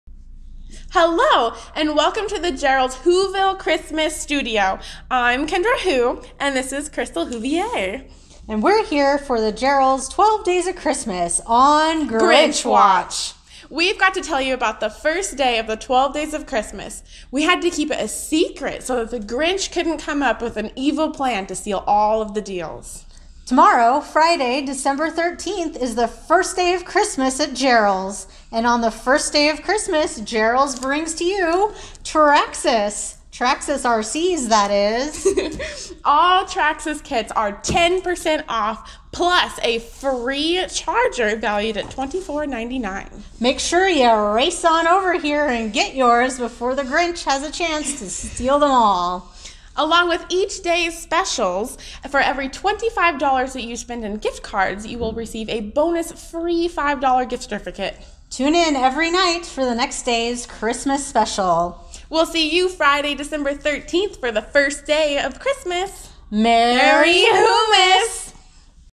Jerrol's 1st Day of Christmas 2019
On the 1st day of Christmas, Jerrol's brings you Traxxas Kits!